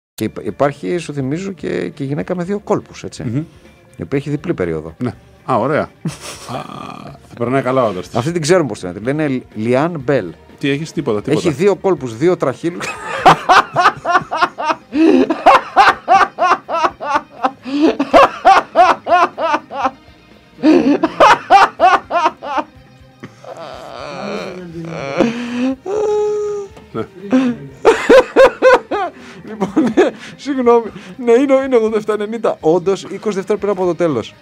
"- Τι έχεις; Τίποτα, τίποτα!"
Καίριο σχόλιο Ψηλού για την γυναίκα με δύο αιδοία, καίρια αντίδραση Αφάνταρου